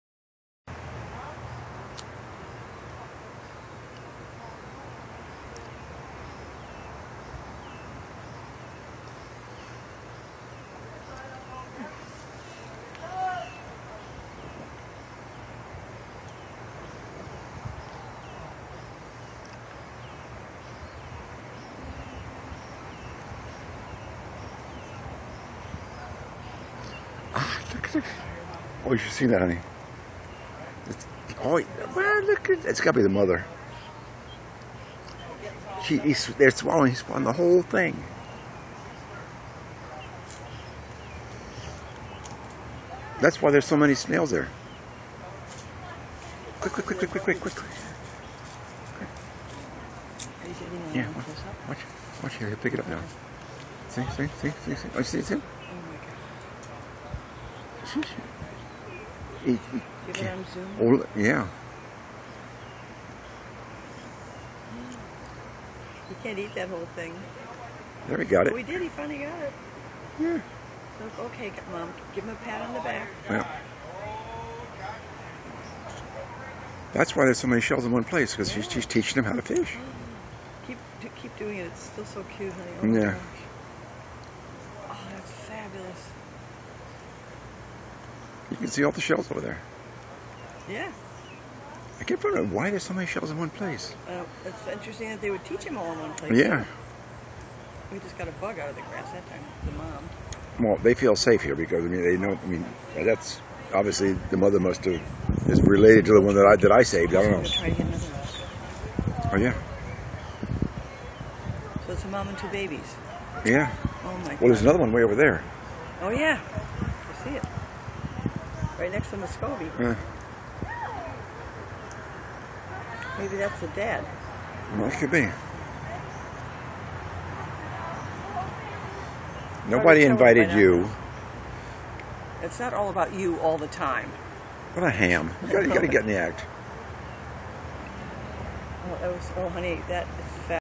Limpkin mother teaching young how to eat apple snails1]
Limpkins are fresh water birds that eat mainly apple snails.  I first met her or her mate a few years ago when she perched on the canopy over my boat dock.  she was tangled in fishing line and let me approach her slowly and let me untangle her from the line.  Since then she returns frequently and deposits empty apple snails on my grass, i guess, as repayment.  I take the shells and recycle them into candle holders, or combine them with driftwood and sea shells to create decorative sculptures .  This is the first time I've seen her with her 2 youngsters in my yard teaching them how to catch and eat apple snails.  When they are finished, there may be as many as six to nine empty snail shells in my yard.